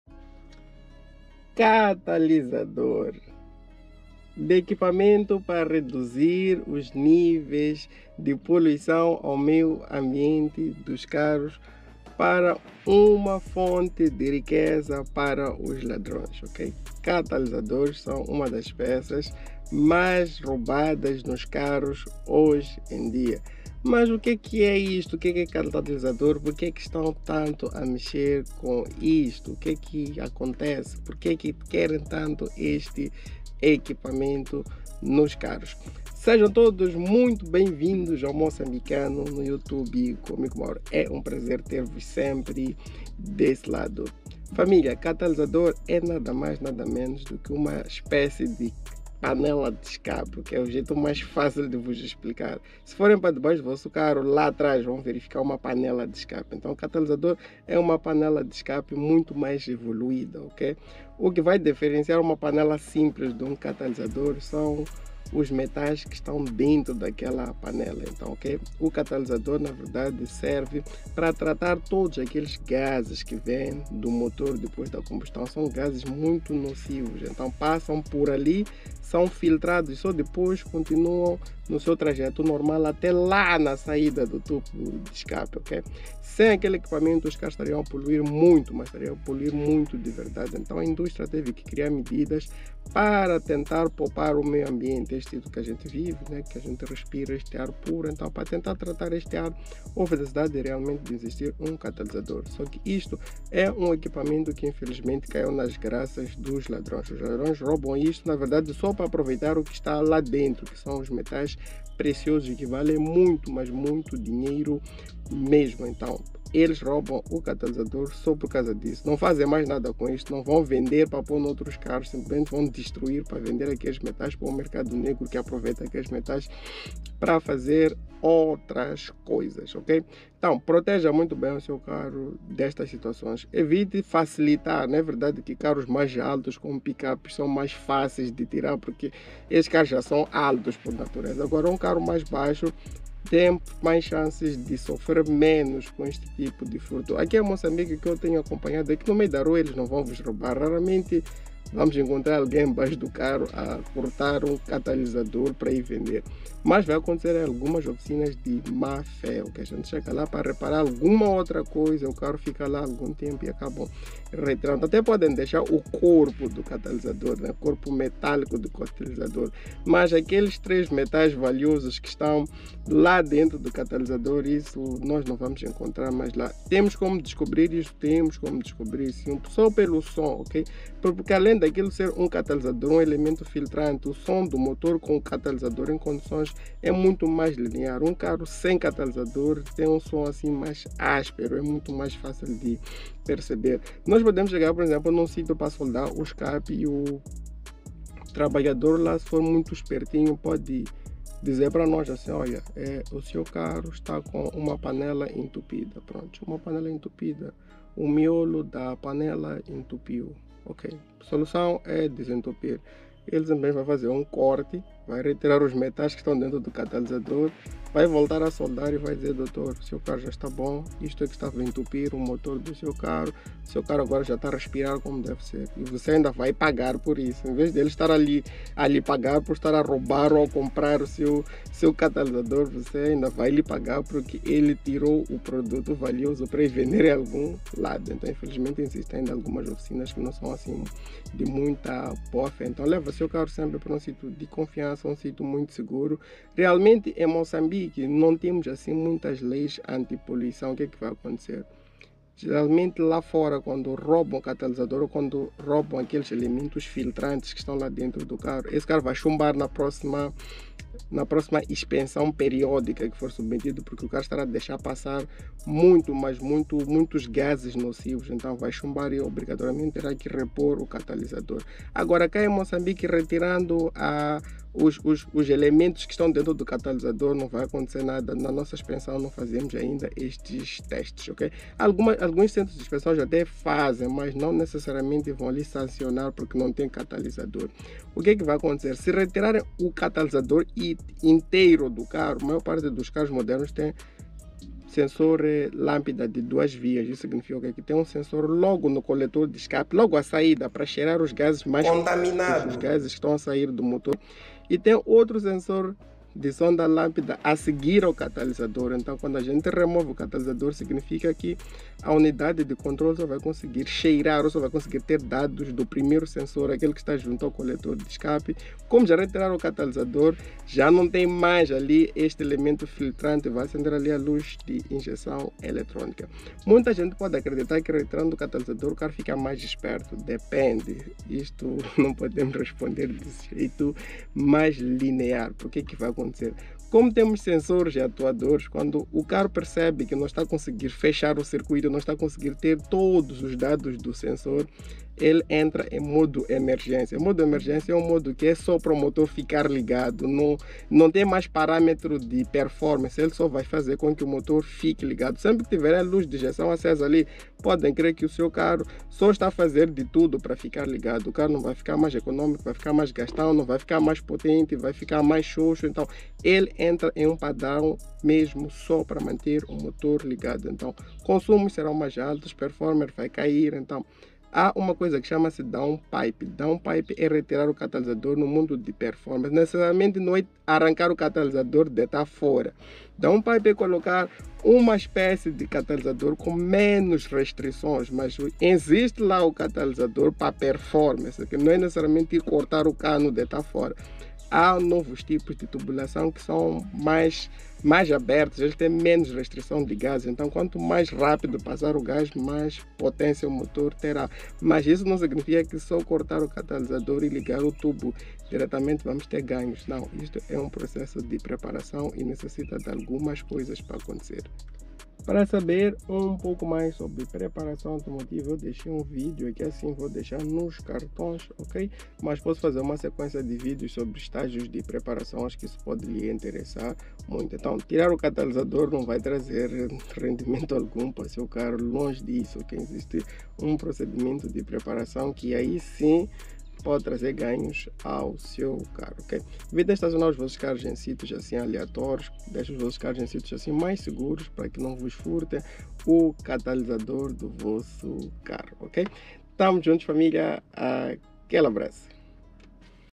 Furto de Catalisadores Em Moçambique | Função Dos Catalisadores
E ai família, tudo bem convosco? Comigo tudo bem.

Neste episódio trago vos este tema bastante interessante no nosso mercado. Desde o surgimento da injeção eletrônica, vários equipamentos ligados a esta tecnologia começaram a aparecer. Um deles é o catalisador. Mecanismo que prometia fazer tratamento dos gazes pós combustão para diminuir sua nocividade para o meio ambiente.

Infelizmente este componente, está sendo alvo dos ladrões devidos os metais nobres que estão contidos no interior do catalisador.